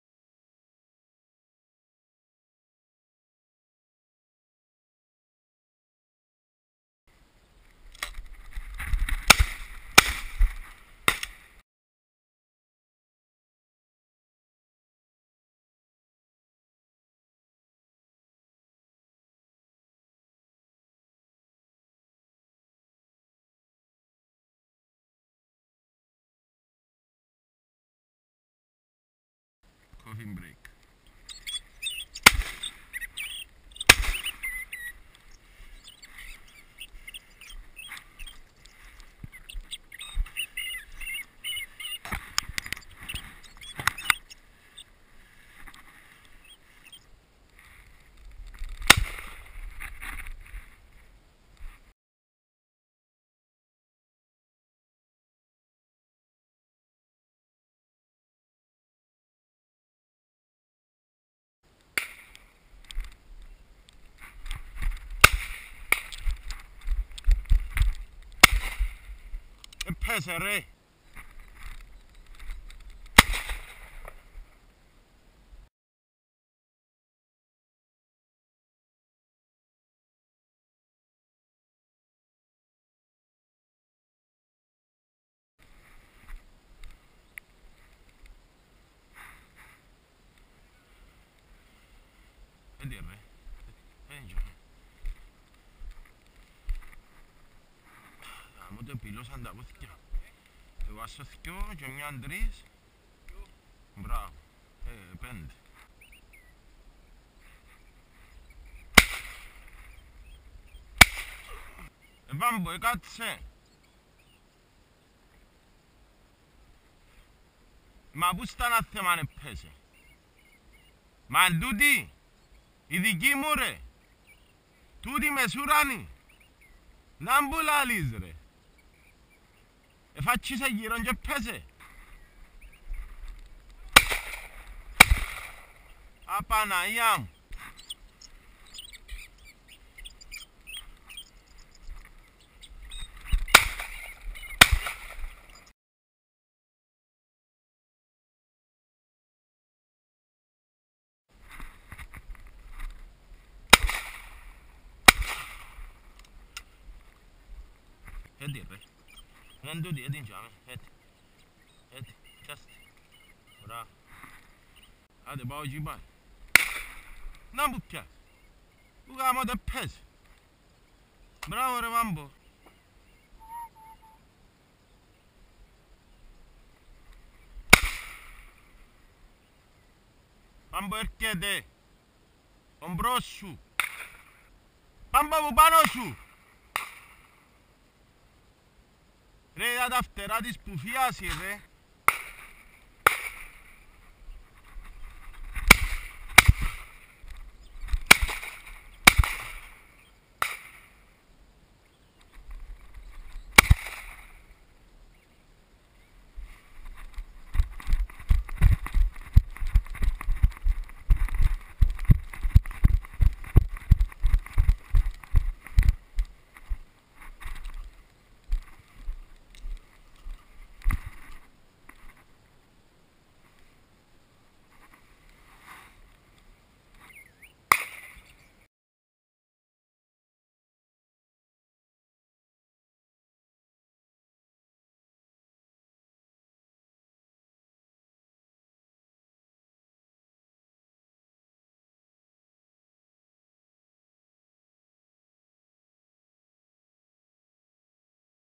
Κυνηγι τσιχλας τον Γεναρη στη κυπρο 2015 thrush hunting in winter cyprus
filoi mou lupamai an i eikona den einai ipsulis evkrinias. me tin nea kinigetiki sezon tha epaneltho me kaliteri camera
hunting with a winchester midnight select 12Ga